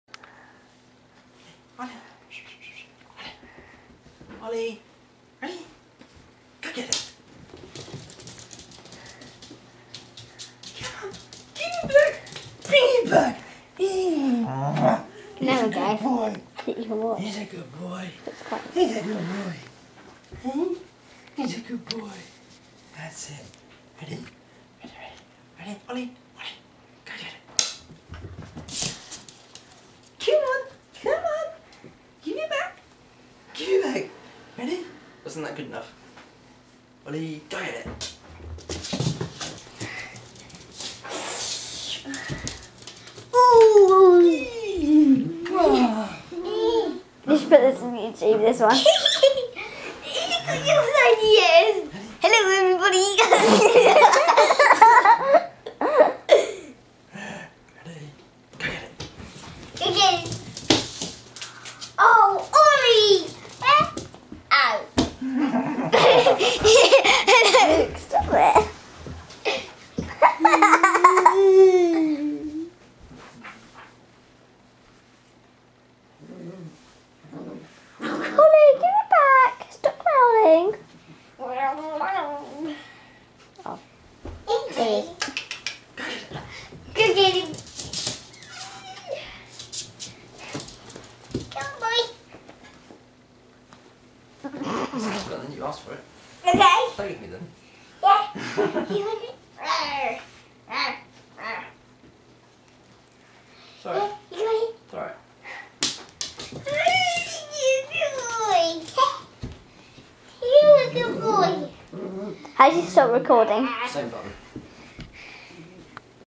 Ollie and Pooh bear.mpg
This is Ollie our 10 week old cocker spaniel puppy.